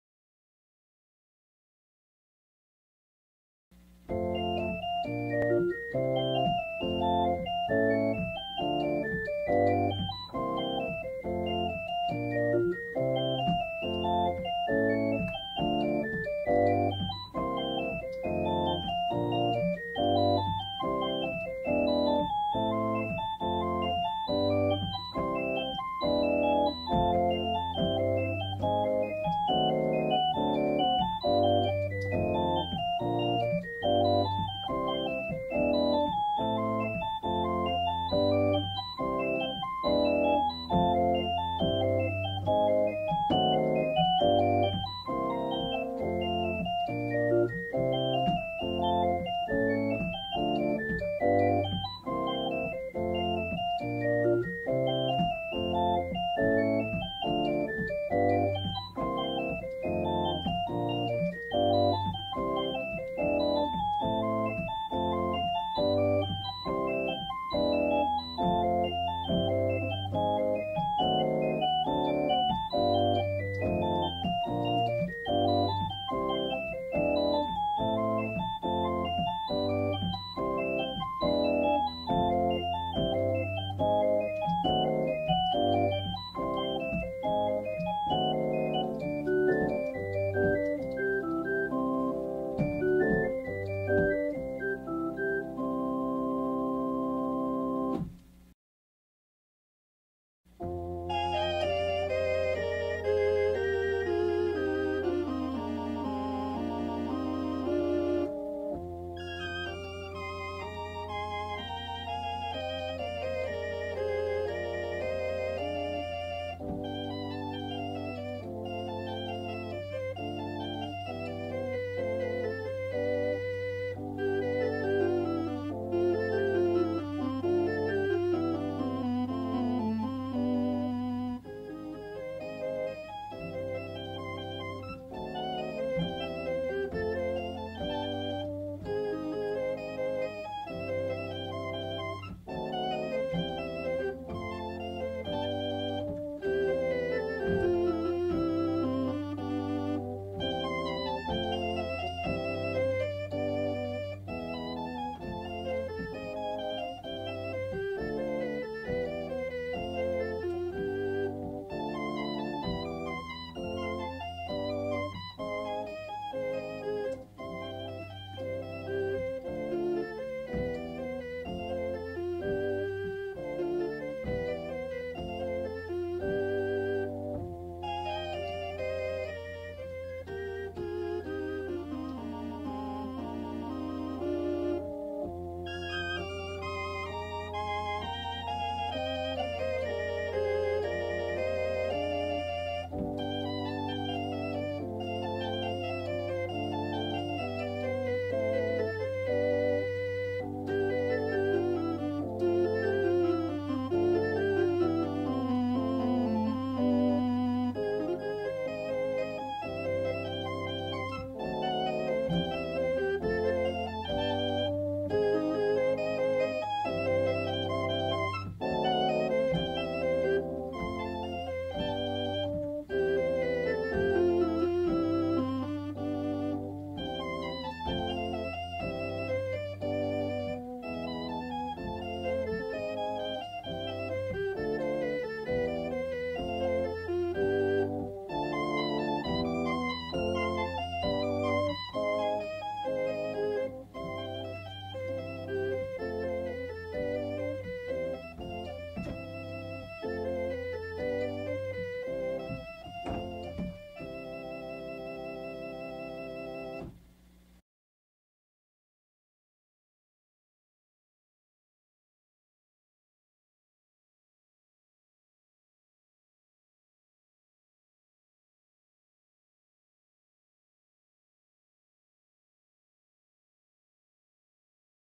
Home Organ 21
This video series features music composed and copyrighted by Charles W. Ragsdale. Ragsdale is the organist (and took the photos; the organ is a Wurlitzer 4500). Movies are also copyrighted by Ragsdale, and I hope you enjoy.